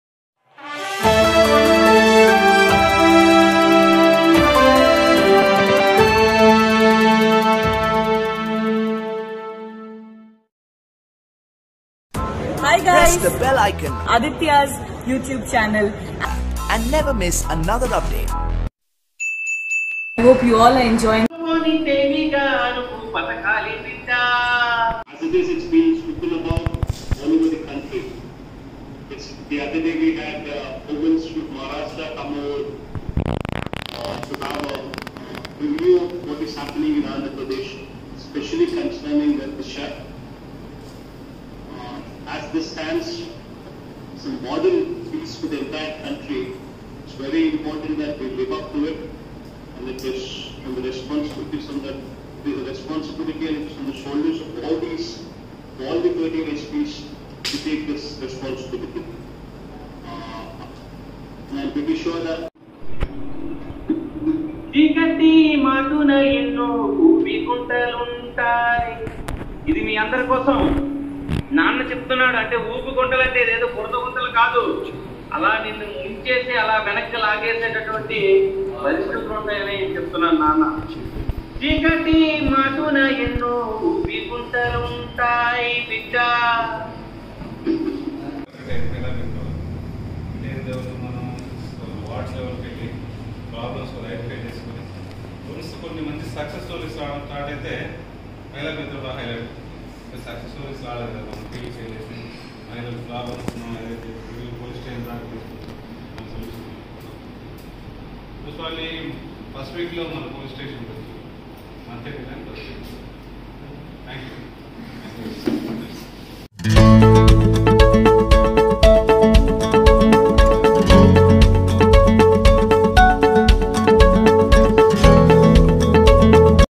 దిశ పోలీస్టేషన్ పై వీడియో కాన్ఫరెన్స్ లో పాల్గొన్న ఎస్పీ మహిళా మిత్రలు
జిల్లా పోలీసు కార్యాలయం మచిలీపట్నం విజయనగరంజిల్లాలో దిశ మహిళా పోలీస్ స్టేషన్ ప్రారంభోత్సవ కార్యక్రమానికి రాష్ట్ర ముఖ్యమంత్రి శ్రీ వై.ఎస్.జగన్మోహన్ రెడ్డి గారు హాజరయ్యి , దిశ పోలీస్ స్టేషనును ప్రారంభించారు. ఈకార్యక్రమనికి సంభందించి నిర్వహించిన వీడియో కాన్ఫరెన్స్లో జిల్లా ఎస్పీ శ్రీ యం.రవీంద్రనాథ్ బాబు ఐపిఎస్ గారు,  ఏఎస్పి శ్రీ యం. సత్తి బాబు గారు పాల్గొన్నారు. పట్టణంలో గల వివిధ వార్డు సచివలయాల  నుండి మహిళ సంరక్షణా పోలీసులు, మహిళా మిత్ర కోఆర్డినేటర్లు, మహిళ మిత్ర  కమిటీ సభ్యులు, SCST సెల్ DSP ఉమామహేశ్వరరావు గారు పాల్గొన్నారు.